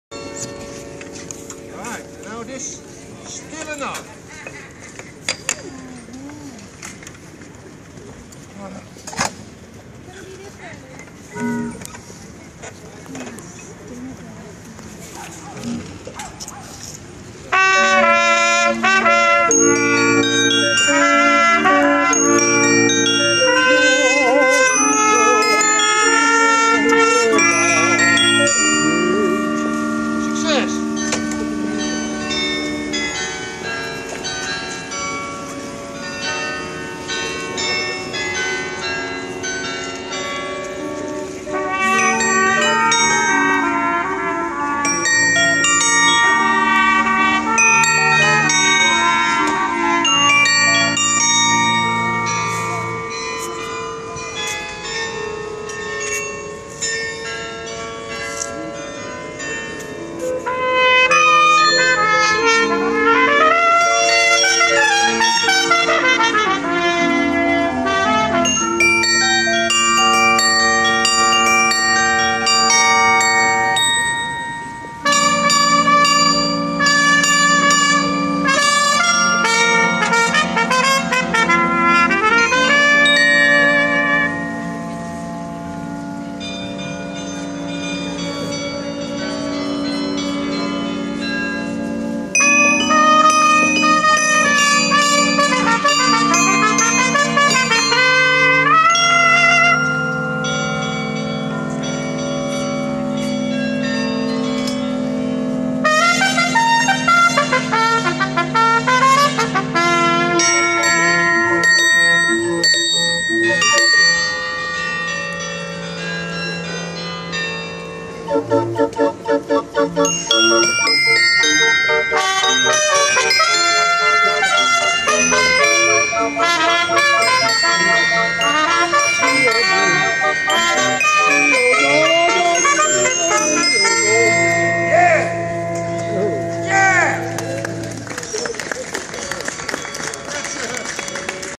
Ride Along with Q - MUZIEKBOOT - Music Boat in Amsterdam 12/18/12 - Hand Video by Q Madp
MUZIEKBOOT - Music Boat in Amsterdam 12/18/12 - Hand Video by Q Madp - I shot this while in Amsterdam. In the background you also hear chimes coming from a church tower overlooking this part of the waterway. The bell ringer there is working with this chap in the water After performance, he would use a fishing rod with a shoe attached to collect tips from onlookers.